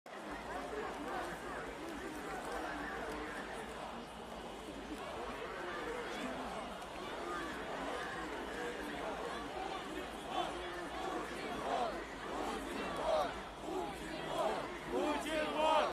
لماذا اختار أنصار نافالني فُرش المرحاض شعارا لاحتجاجاتهم ضد بوتين؟…
فيديو القصر الفاخر الذي نشر على مواقع التواصل الاجتماعي حصل على أكثر من 86 مليون مشاهدة منذ نشره على موقع يوتيوب. وينفي الكرملين أن تكون علاقة الرئيس بوتين بملكية القصر